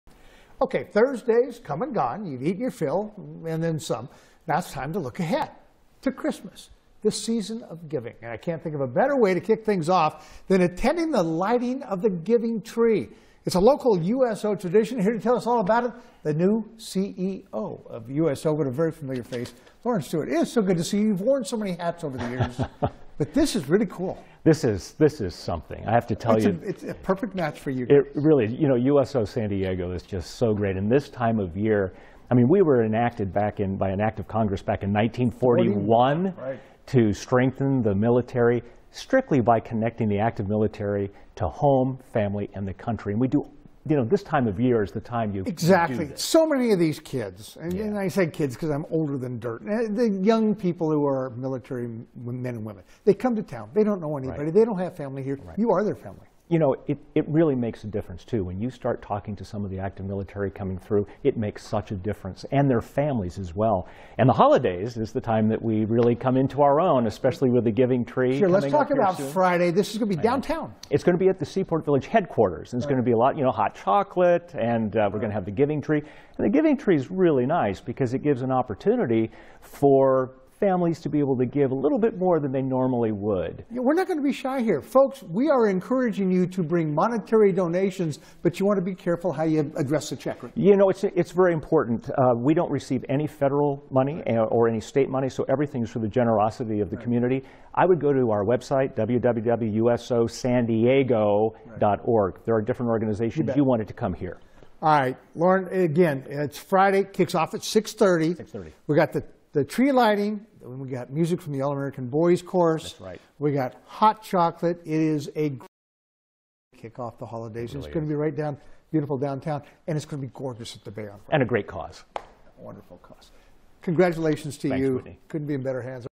USO San Diego on NBC discussing Annual Giving Tree Lighting Ceremony
USO San Diego CEO, Lorin Stewart, talks about the Annual Giving Tree Lighting Ceremony and how USO San Diego connects the military with family during the holidays.